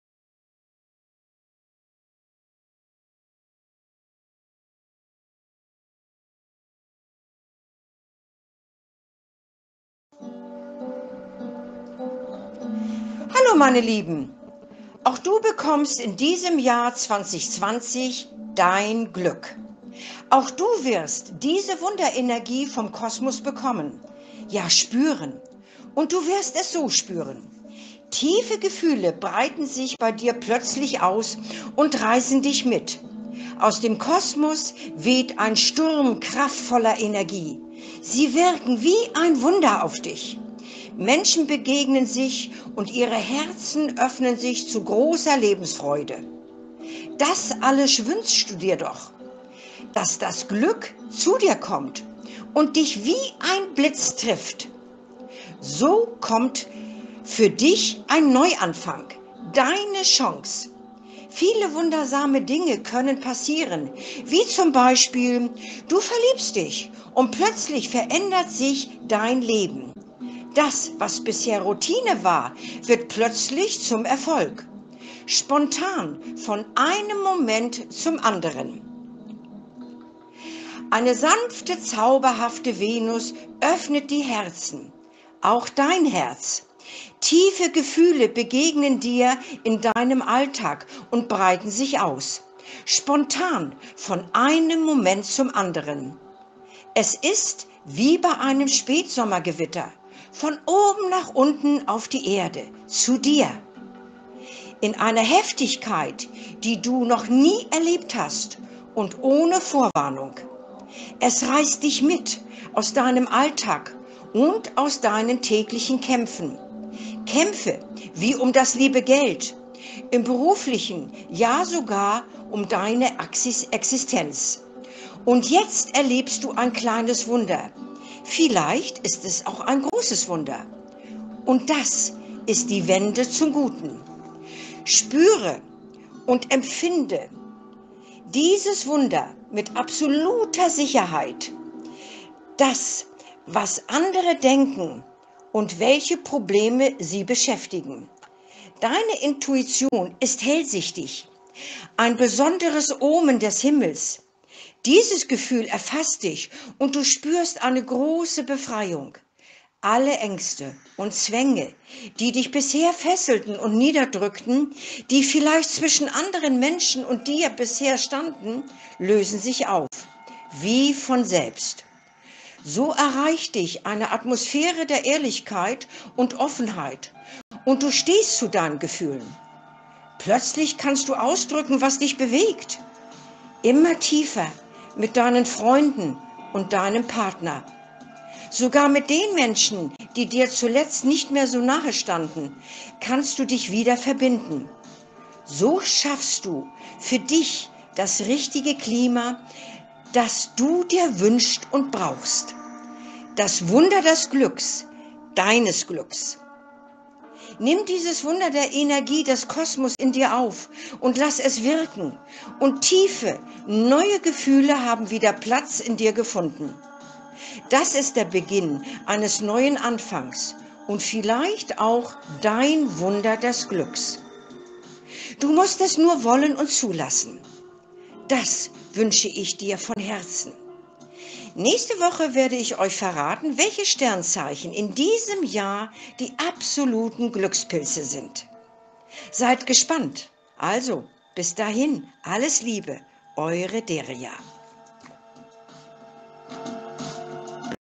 22.01.2020 Dein Glück kommt mit der Wunderenergie des Kosmos
Aus dem Kosmos weht ein Sturm von Energie zu uns . Man wird es spüren - auch DU wirst diese Wunderenergie spüren. Wie es sich bemerkbar macht, erkläre ich .

Ich biete an:
*** Hellsichtiges Kartenlegen
*** Astrologie
*** Channeling
*** Coaching
*** Seminare

Seminare:
 *** Engelseminare

Herzlichst
  Deria